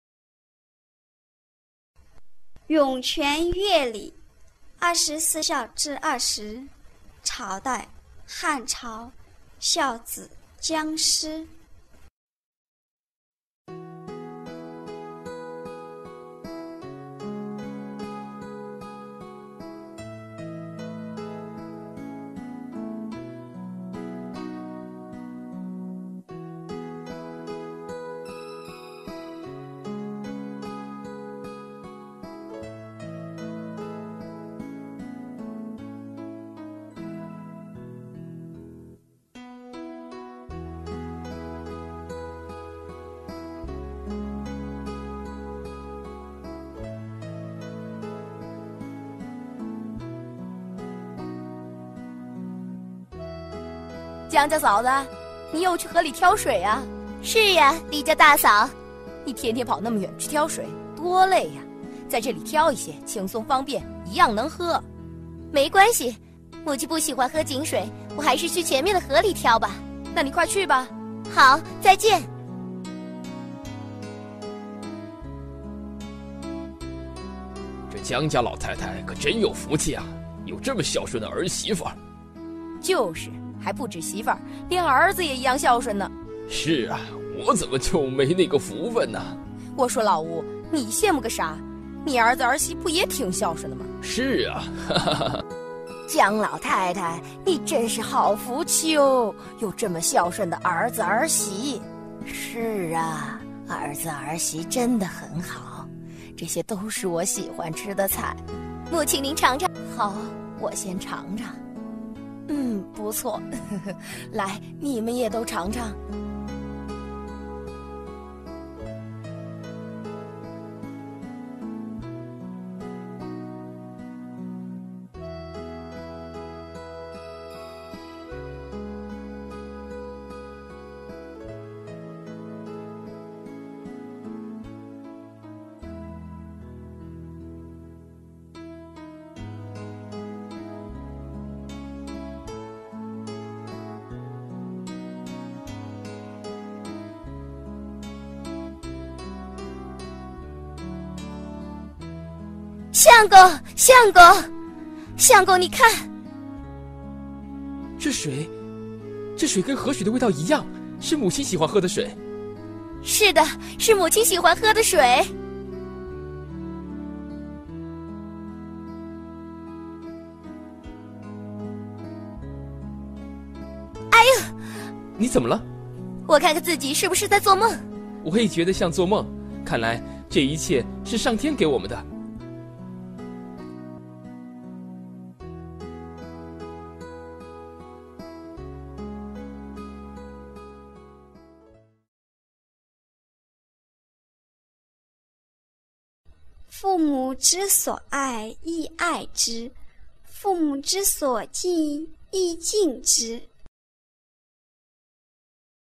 二十四孝：20 湧泉躍鯉 Carps Jumping Out of a Gushing Stream
二十四孝的故事 24 Stories of Filial Devotion
朝代：漢朝 Dynasty: Han
孝子：姜詩 Filial son: Jiang Shi